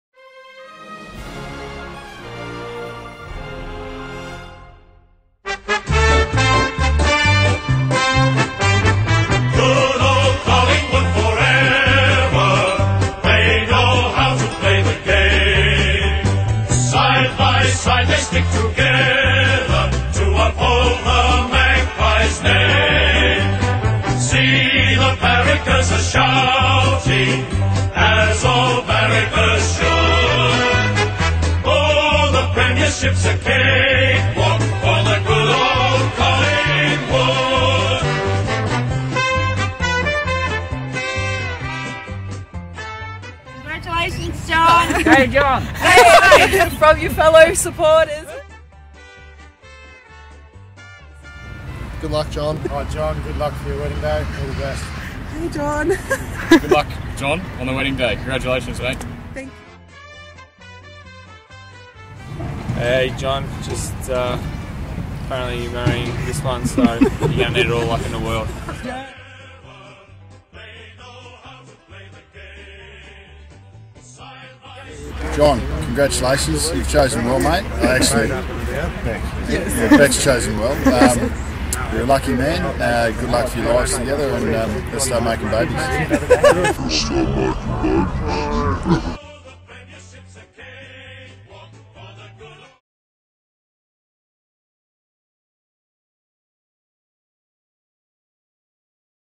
Best wedding surprise for Collingwood Supporter Groom
Wife surprises husband on wedding day with messages from Collingwood players and coach!